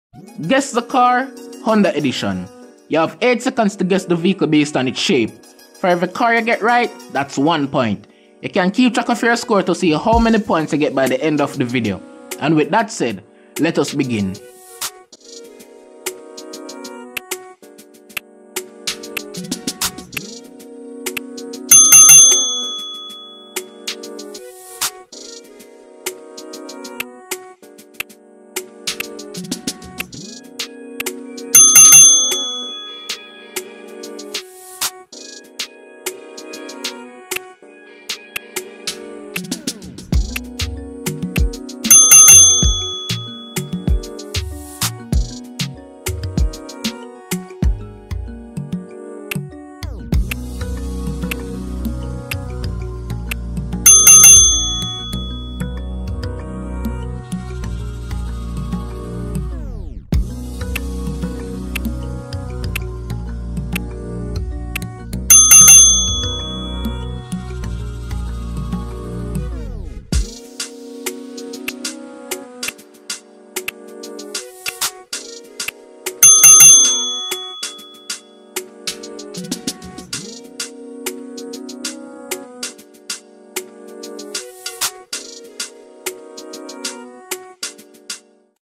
can you guess the car by it's body shape?
A fun little game of guess the car, Honda edition.